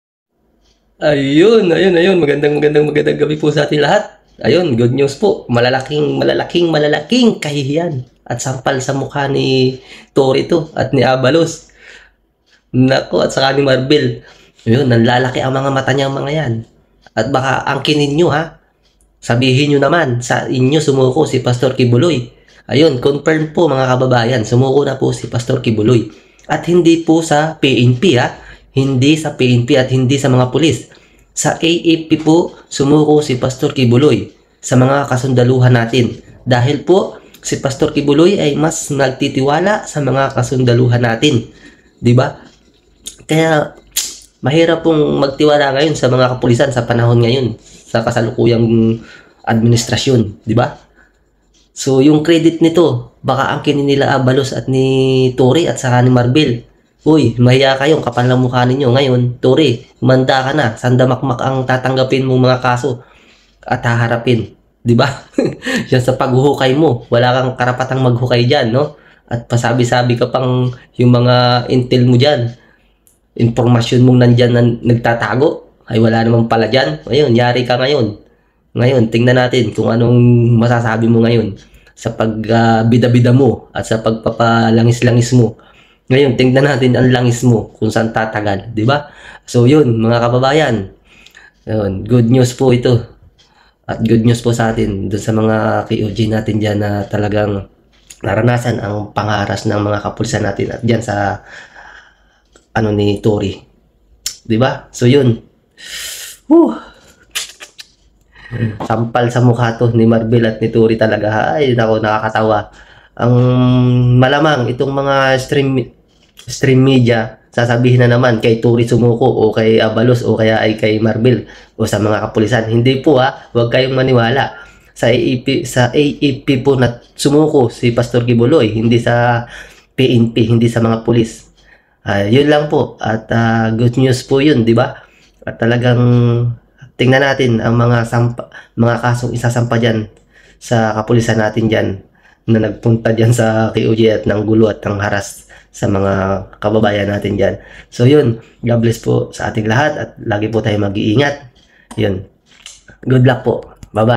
CONFIRM pastor QUIBOLOY Sumuko na TORRE, ABALOS MAKUKULONG!
Kanino mga ba sumuko si pastor QUIBOLOY alamin!